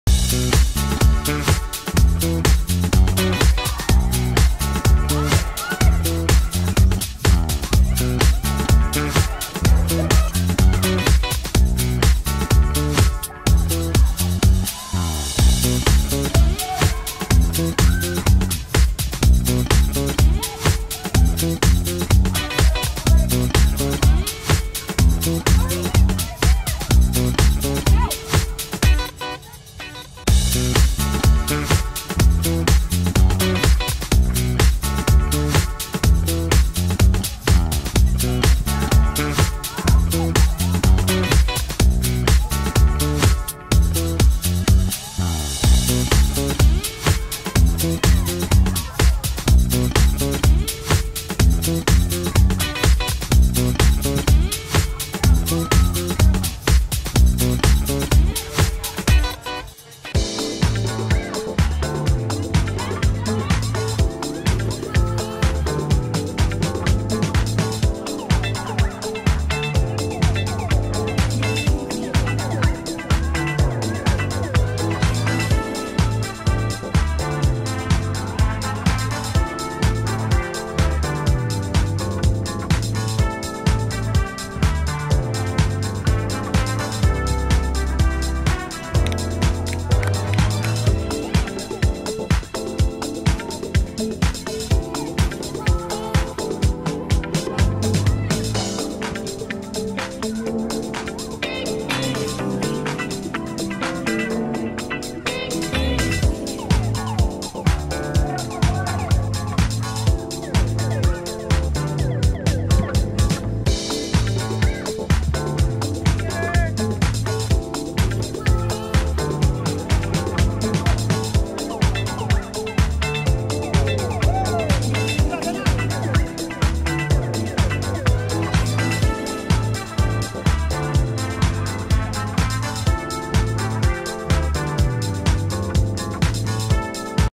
MN Eclipse Showcase Highlights
These are clips of me that I got from the MN Eclipse Showcase last weekend.

2023 Goalkeeper for Manitou FC Sambas,
U-18 Premier 1